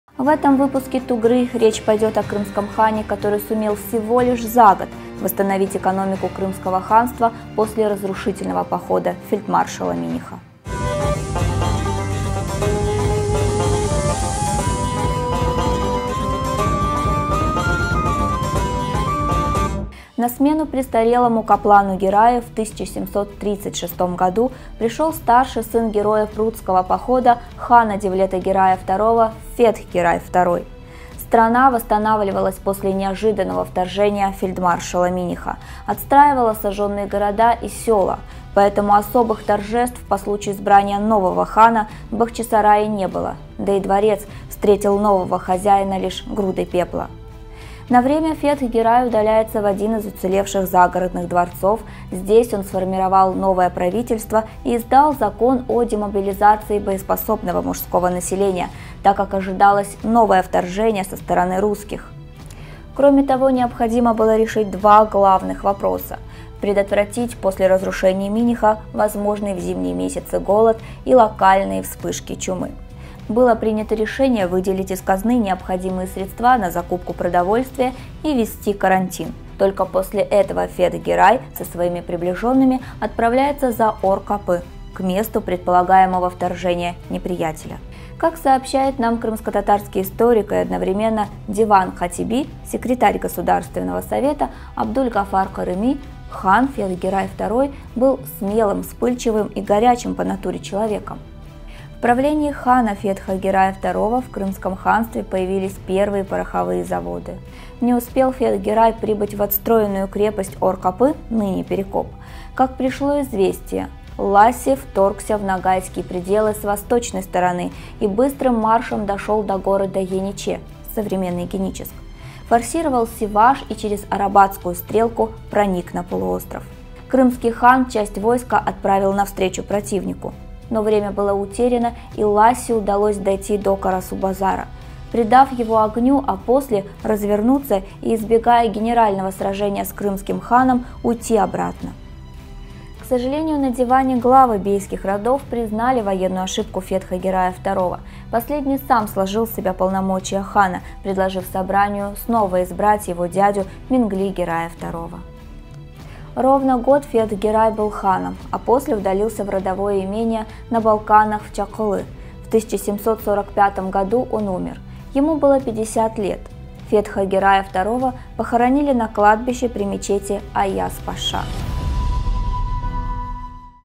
Видеоблог «Tugra»: Фетх Гирай II хан
Новая серия видеоблогов рассказывает о династии крымских ханов Гираи. Кто были эти люди, как пришли к власти и какой след оставили после себя. В центре каждой серии – судьба одного из героев.